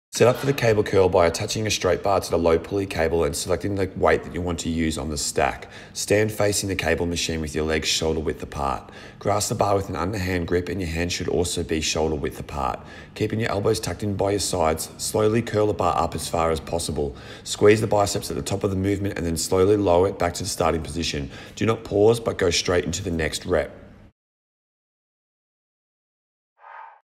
Cable Straight Bar Curls
Cable Curl Instructions
Set up for the cable curl by attaching a straight bar to the low pulley cable and selecting the weight you want to use on the stack.
Stand facing the cable machine with your legs shoulder width apart.
Grasp the bar with an underhand grip (palms facing up), and your hands about shoulder width apart.
Keeping your elbows tucked in by your sides, slowly curl the bar up as far as possible.
Squeeze the biceps at the top of the movement, and then slowly lower it back to starting position.
Do not pause, but go straight into the next rep!
​Cable Curl Tips:
Keep your elbows tucked in by your sides and don't allow them to come forward as you curl the weight up.
Don't "rest" at the bottom of the movement. When you complete a rep move straight into the next rep!
Keep the rep timing slow and control the weight as you lower it.